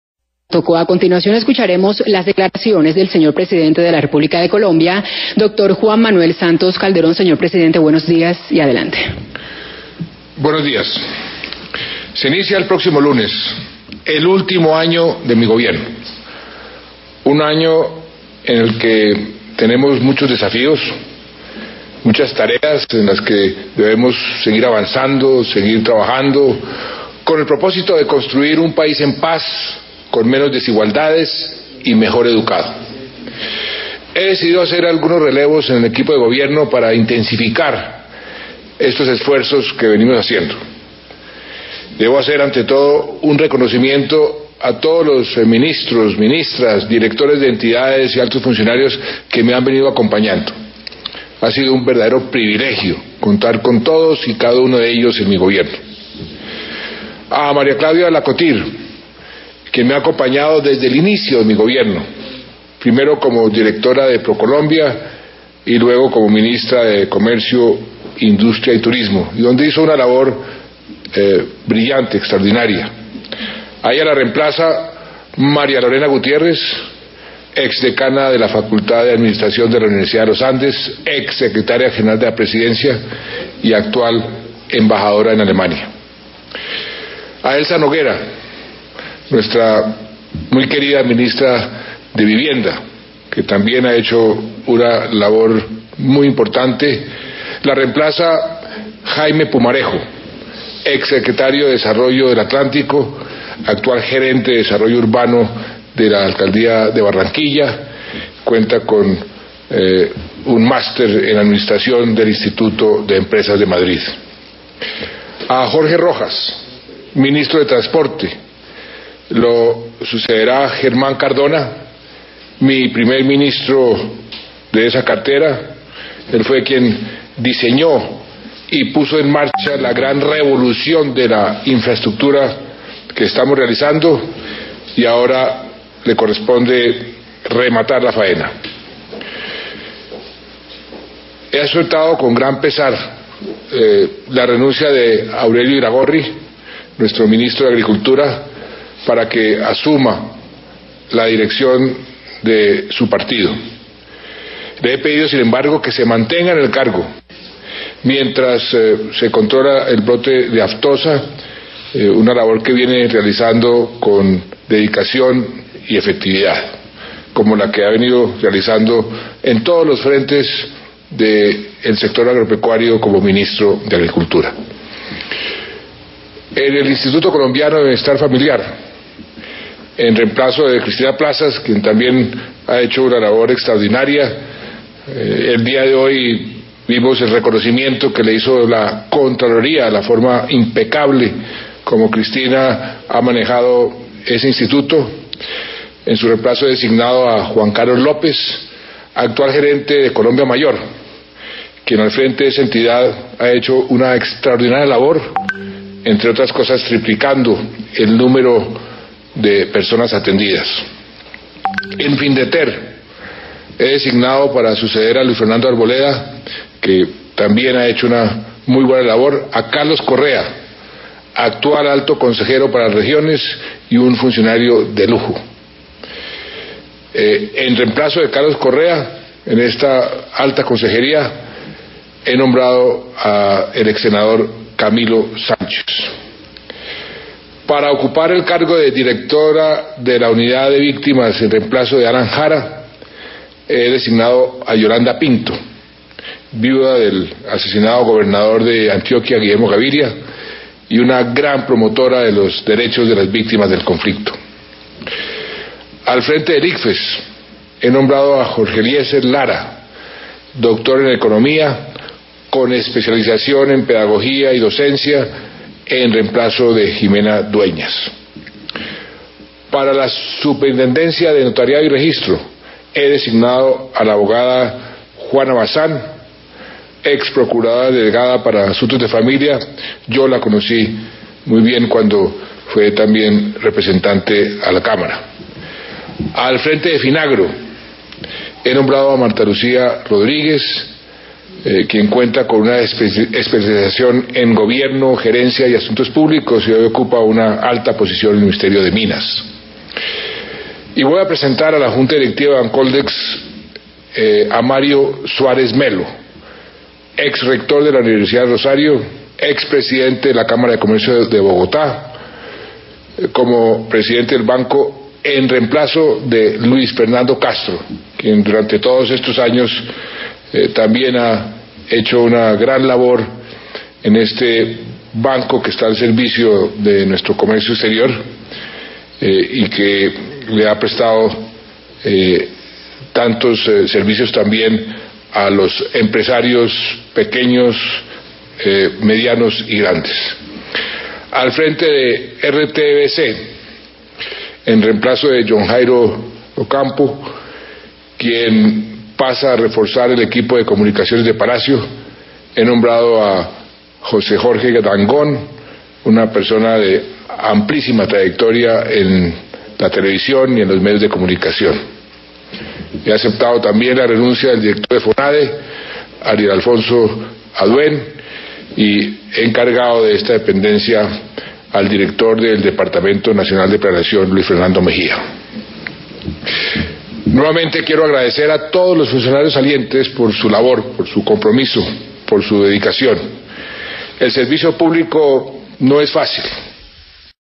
Santos anuncia cambios en cuatro ministerios - Agosto 1, 2017
@Horadelaverdad - Escuche el programa en directo de Lunes a Viernes de 6 a 10 a.m. y visite la página de facebook